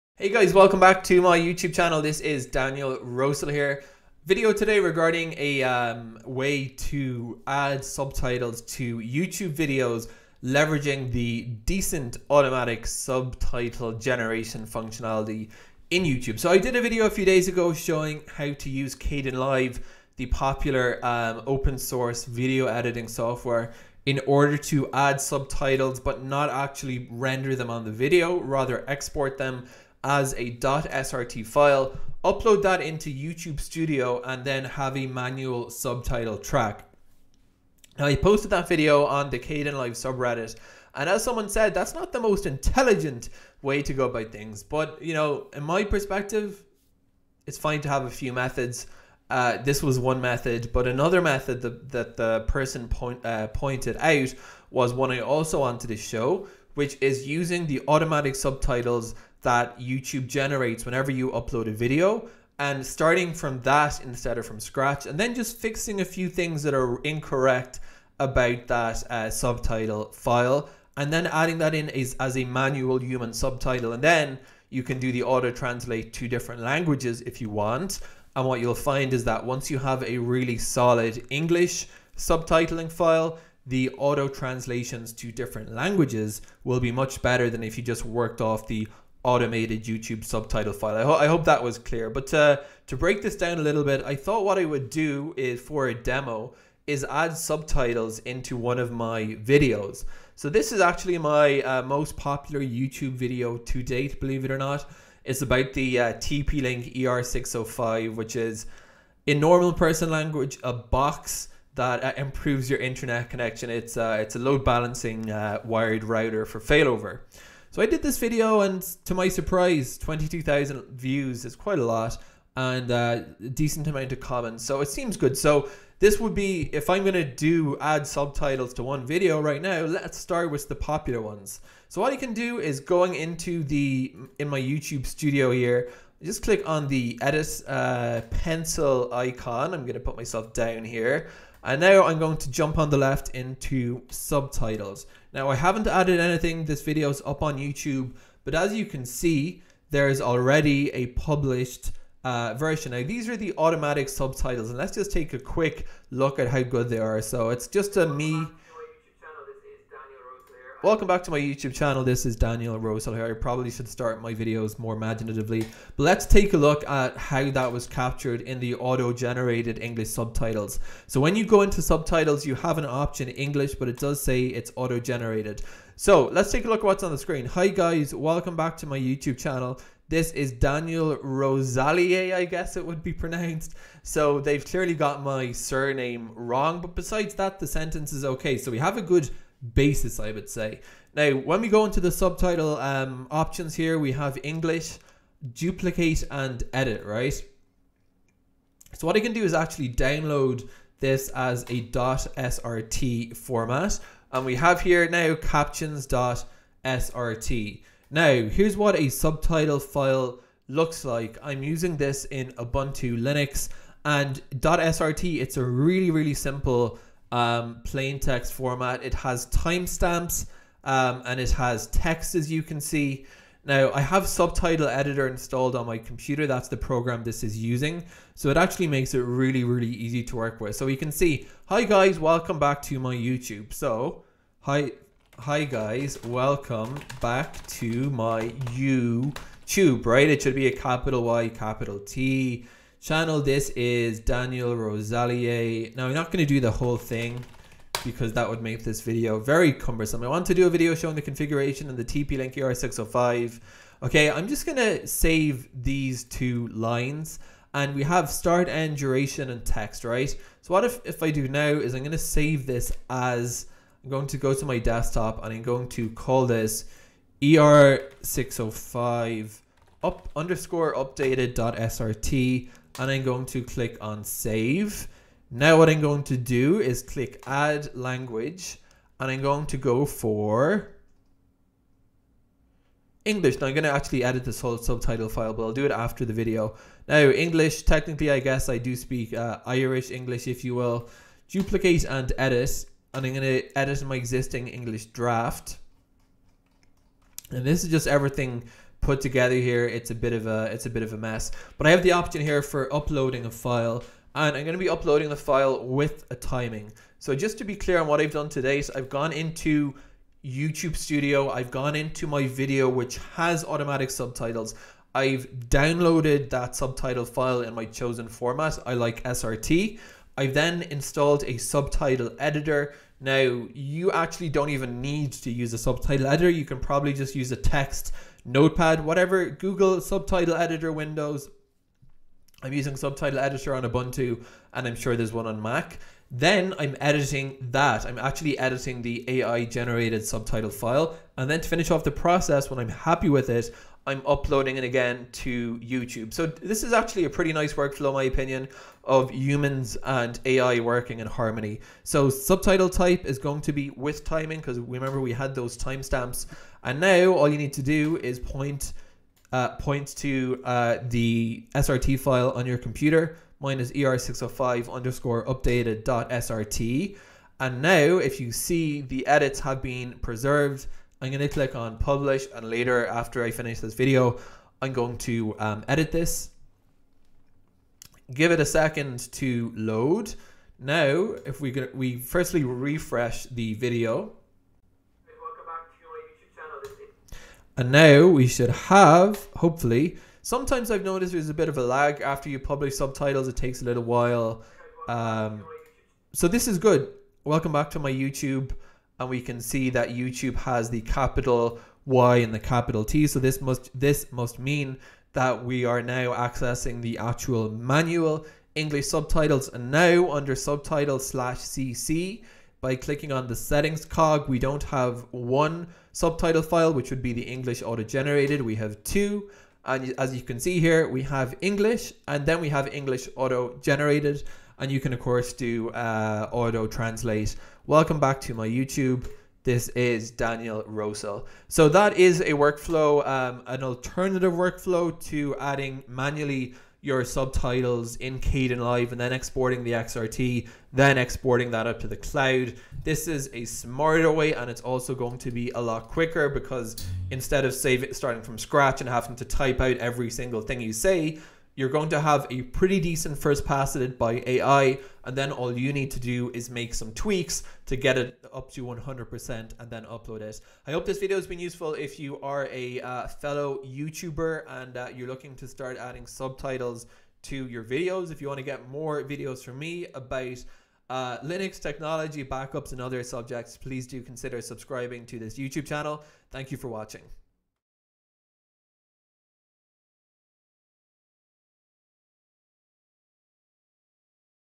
How To Edit YouTube Autogenerated Subtitles (In A Desktop Subtitle Editor)
How to use a desktop subtitle editing software to improve the automatically generated subtitles that are added by YouTube whenever you upload a video.